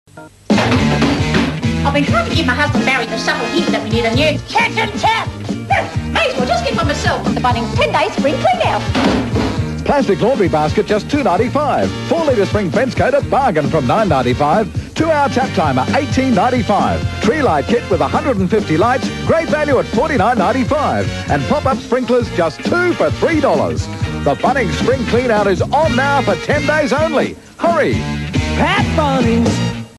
TVC - Bunnings '10 Day Spring Cleanout' (1995)
Another classic Bunnings TVC predating 'Everyday Low Prices (That's the Bottom Line)'